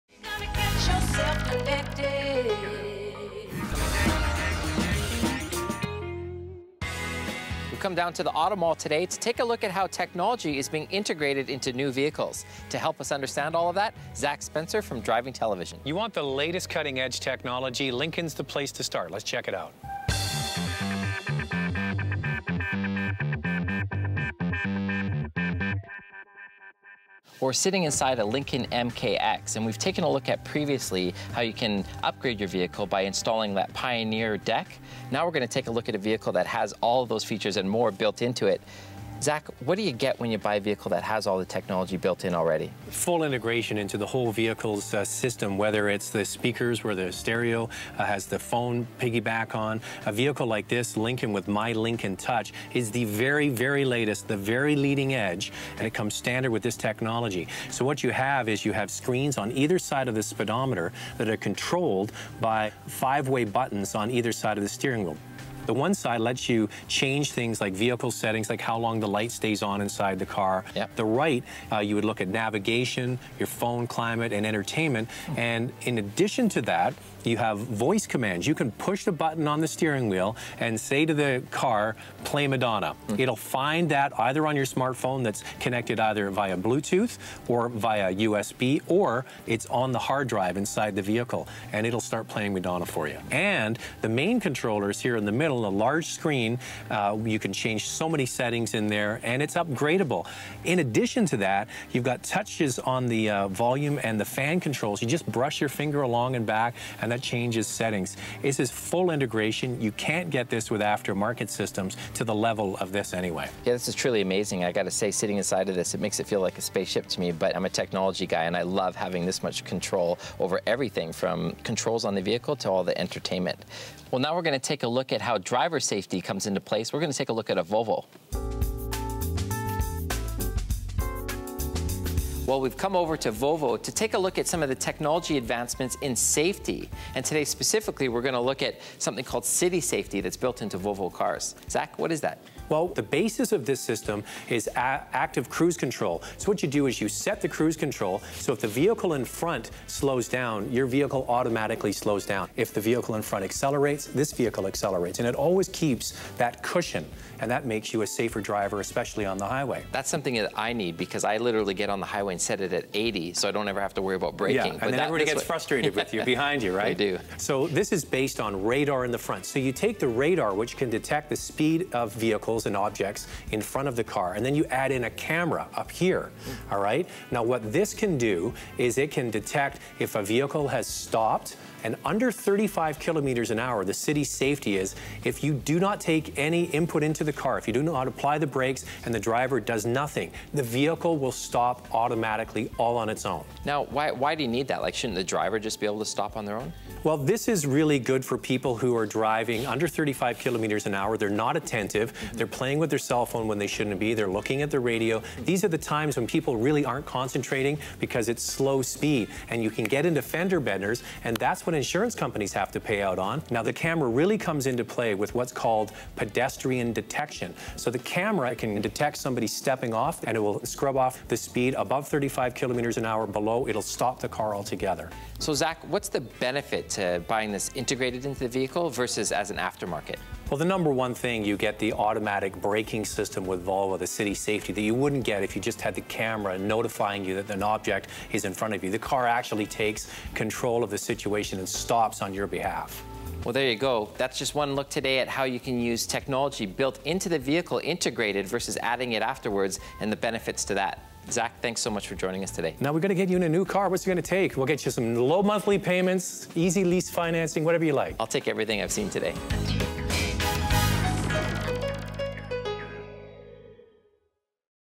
New Car Technology
AJ Vickery talks with Zack Spencer from Driving Television about how technology is being integrated into new vehicles.